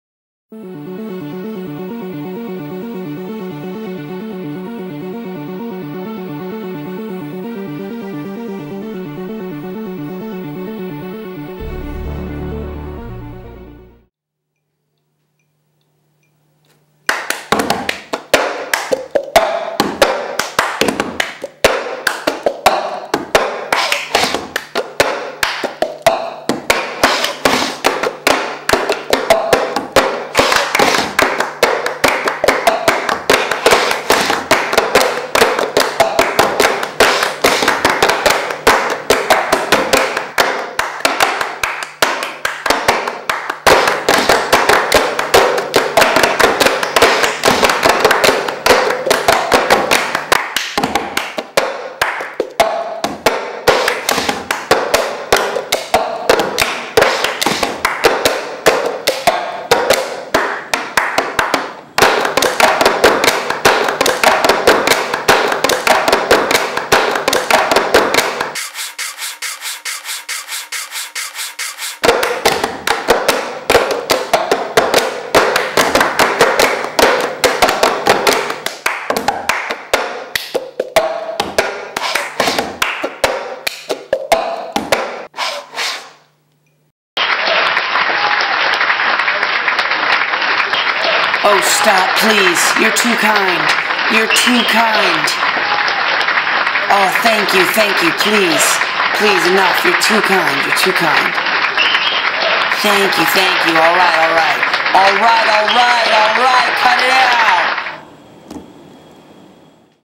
Cup Symphony
Hey guys, this is me making music with cups!...Enjoy!